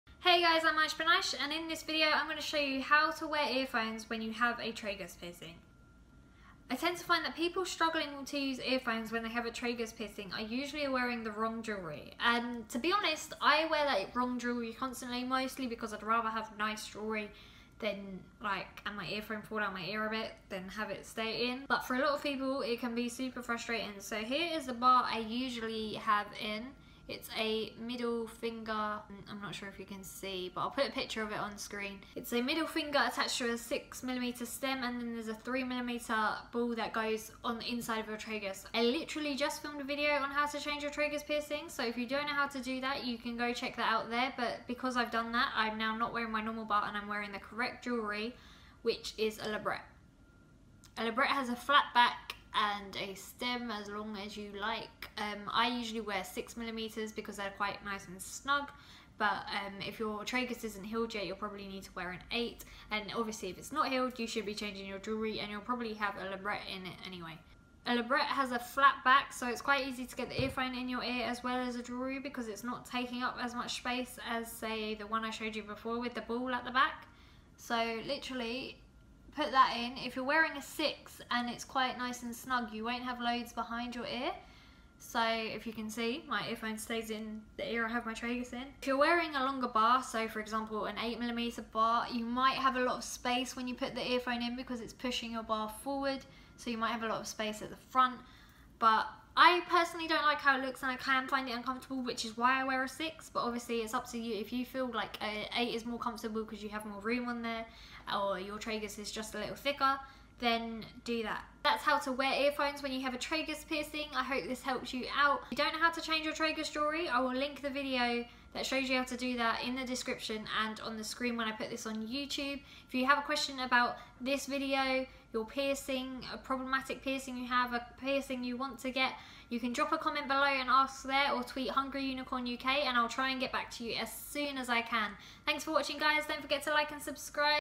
How To Wear Earphones With A Tragus Piercing | Piercing Tutorial
In this video I'll be demonstrating how to wear earphones with a tragus piercing

Sign up to my mailing list for competitions, offers, advice & more